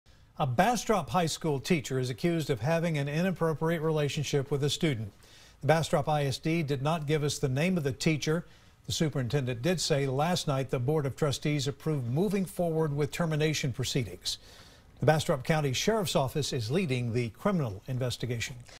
Bastrop High School teacher accused of inappropriate relationship with student
Bastrop ISD did not give the name of the teacher.